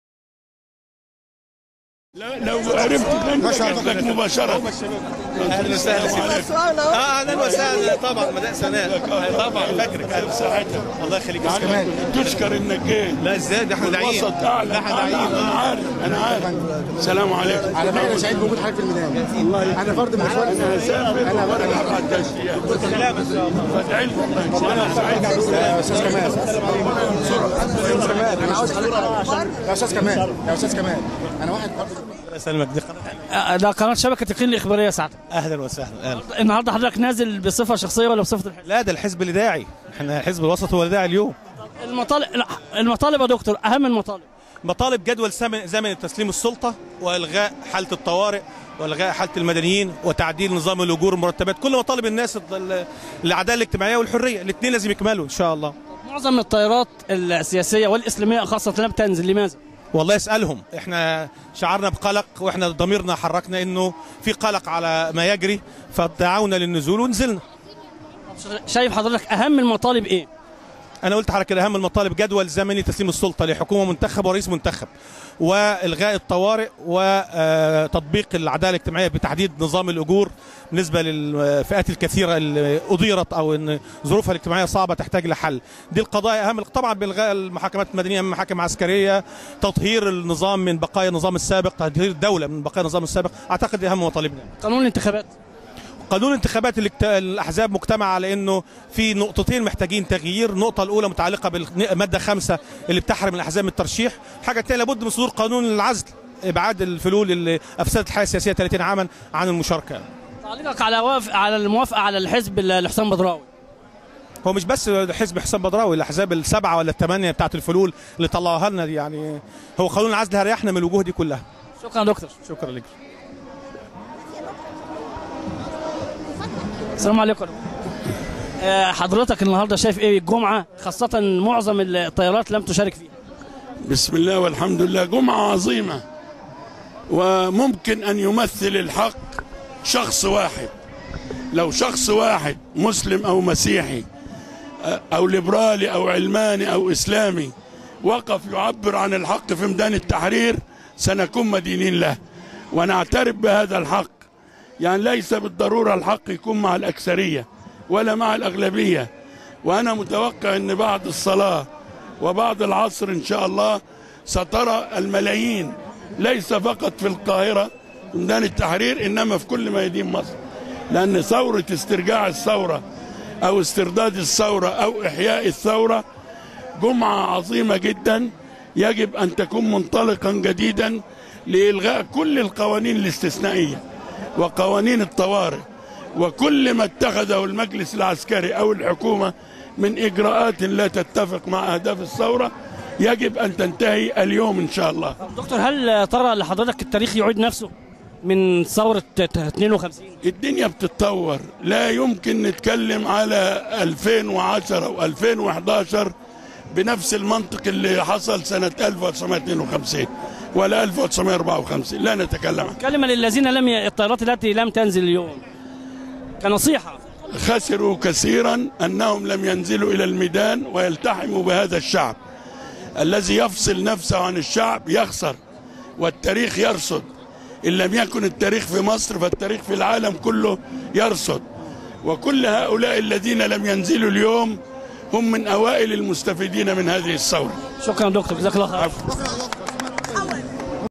تنفرد شبكة يقين لقاء بين أبو العلا ماضى و كمال الهلباوى
لقاء بين أبو العلا ماضى و كمال الهلباوى فى ميدان التحرير فى جمعة 30\9 فى تسجيل مع شبكة يقين الإخبارية